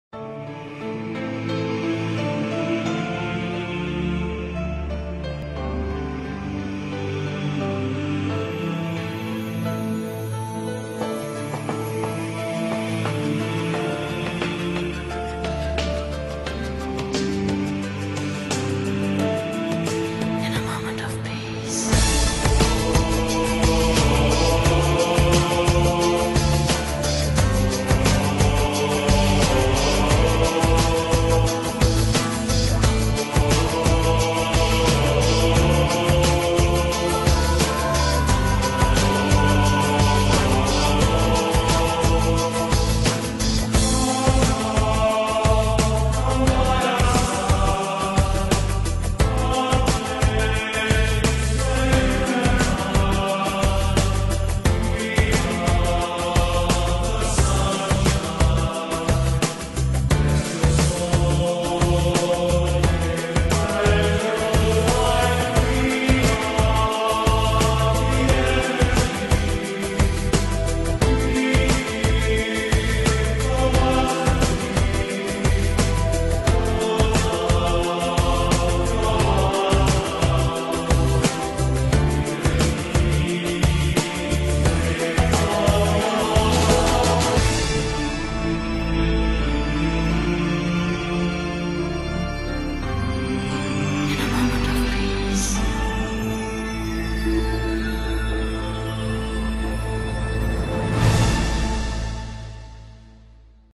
Kispest, Wekerle telep, Kós Károly Tér - Drone felvételen.
Kispest, de inkább Wekerle telep, még inkább a Kós Károly Tér és környéke. -  Kezdetleges Drone videó. Lassan sikerül másik vágóprogramot szereznem :)

Köszi a figyelmet!

J.V.